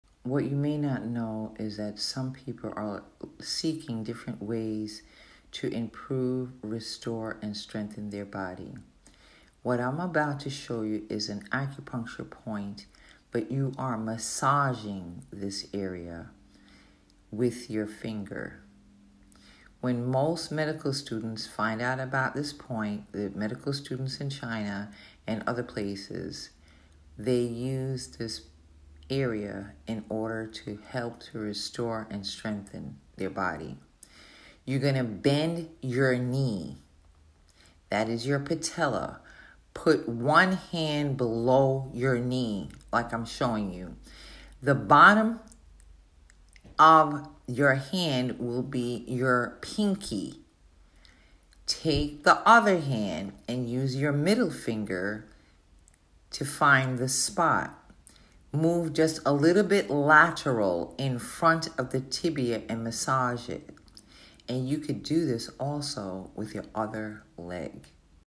Strength and Restore your Body
Over three hundred anatomical acupuncture points are available for treatment of pain and other related ailments. TCM has a long medical history of treating patients for chronic and acute pain and more……
The point described in this video is an incredible healing point you can massage